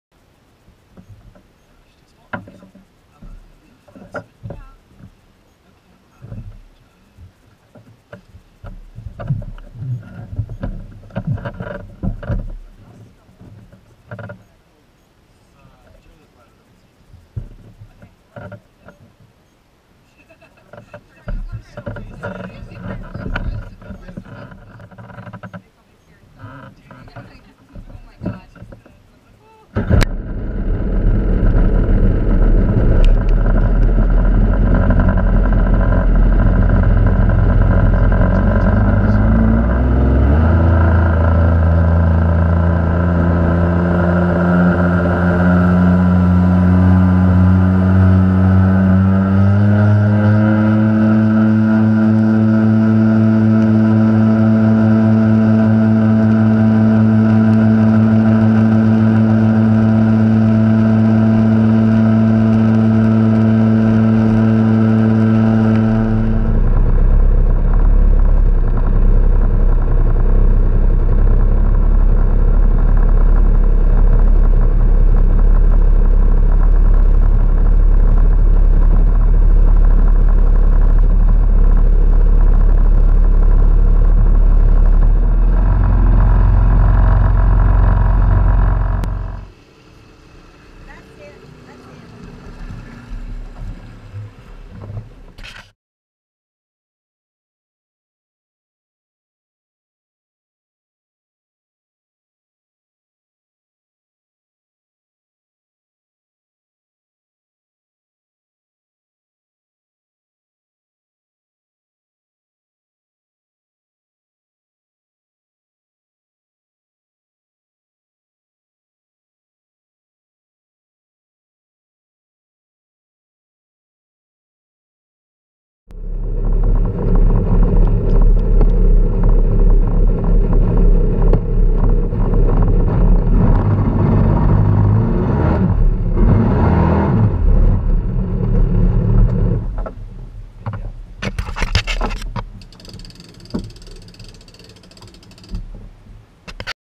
July 4th 2014 JetSkis on Perdido River
Quick Jet Ski ride after the family fish fry